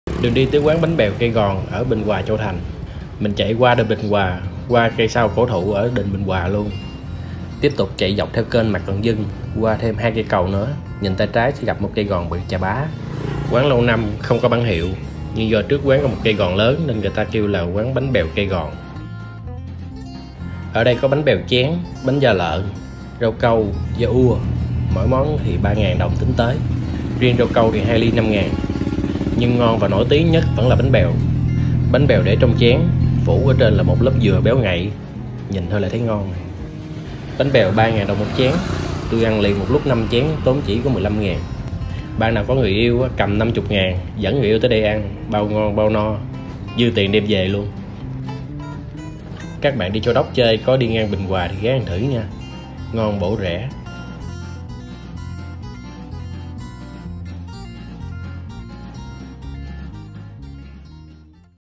Bánh Bèo Cây Gòn ngon bổ rẻ, nổi tiếng nhất Bình Hoà, Châu Thành
Bánh Bèo Cây Gòn ngon bổ rẻ, nổi tiếng nhất Bình Hoà, Châu Thành, An Giang.

Đường đi tới quán bánh bèo Cây Gòn ở Bình Hoà, Châu Thành:

- Hướng vào chợ Bình Hoà, đi cặp kênh Mạc Cần Dưng, qua Đình Bình Hoà khoảng 2km, nhìn tay trái sẽ thấy 1 cây gòn rất lớn, quán Bánh bèo nằm ngay đó.
- Khoảng 2-3h chiều quán mới bắt đầu bán nhé.

Ở đây có bánh bèo, bánh da lợn, rau câu,... nhưng bánh bèo vẫn là món ngon và nổi tiếng nhất. Mỗi món chỉ 3k, riêng rau câu 2 ly 5k. Cái nào cũng ngon, combo 3 món chưa tới 10k.

Có dịp đi ngang Bình Hoà bạn hãy ghé ăn thử nhé, bảo đảm ngon, bổ, rẻ.

(Clip có sử dụng hình ảnh của bạn Phan Điền Viên, Lê Thuý Vy, Hồ Điệp)

©️ Lang Thang An Giang Review